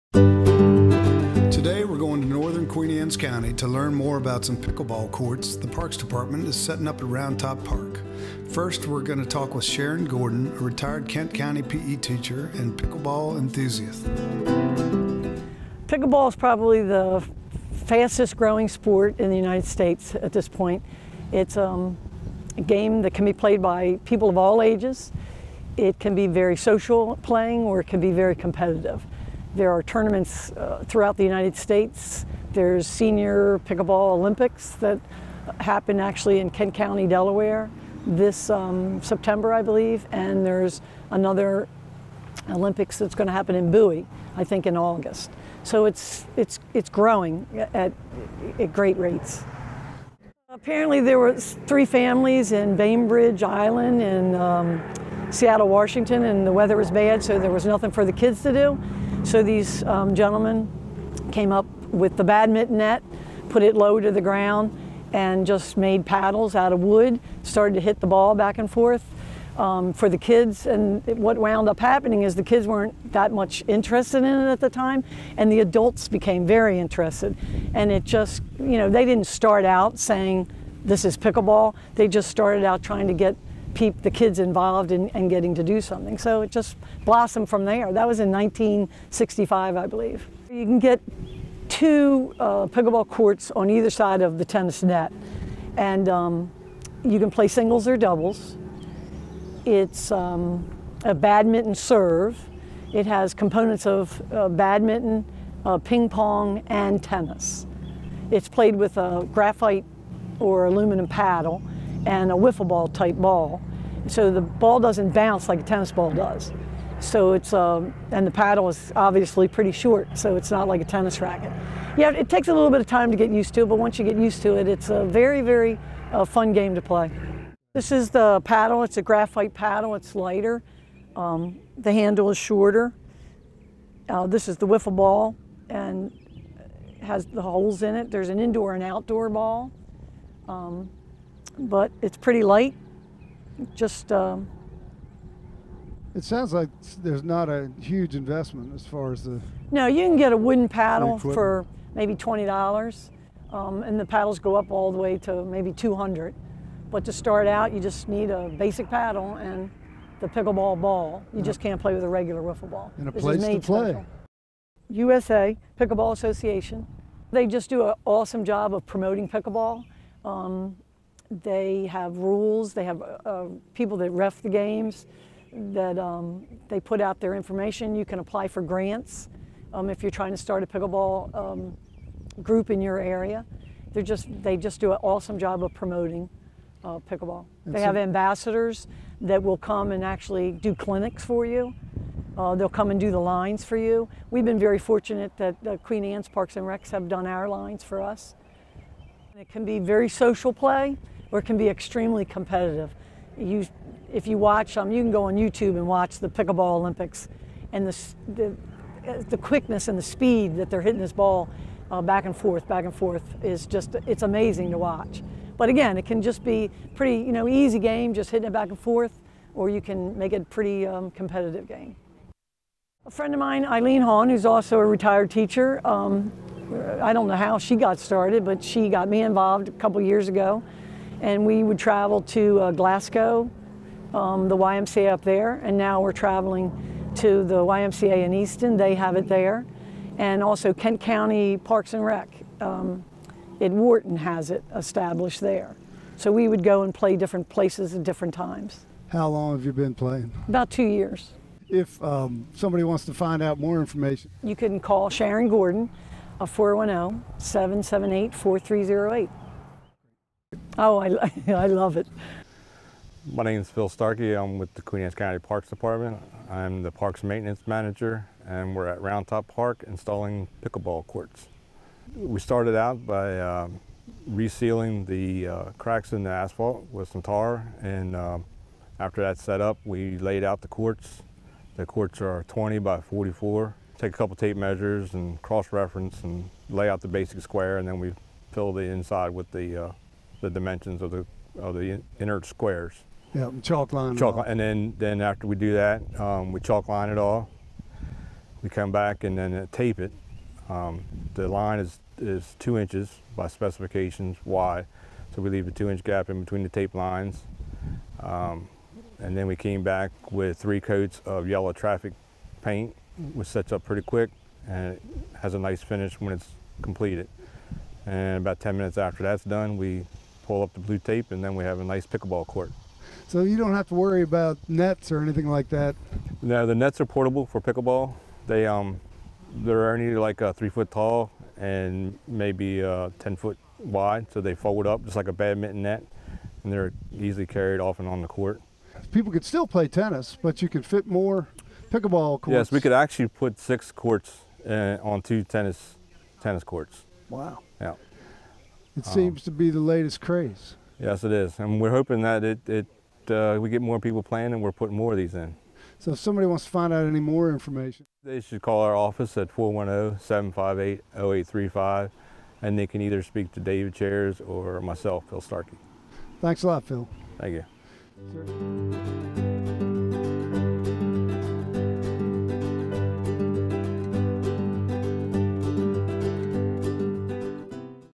Join the New Sport called Pickleball!
Queen Anne's County Parks staff have been busy setting up Pickleball courts. Learn more about the sport and join the fun!